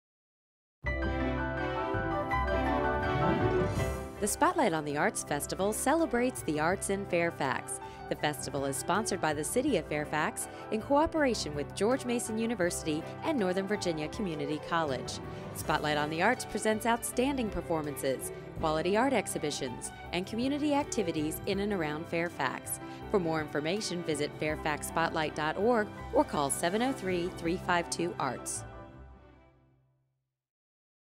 Spotlight on the Arts 2013 Promo
Celebrate the arts in Fairfax with events held throughout the City of Fairfax organized by the Spotlight on the Arts Committee, hosted in part by George Mason University, and Northern Virginia Community College from April 19 through May 6.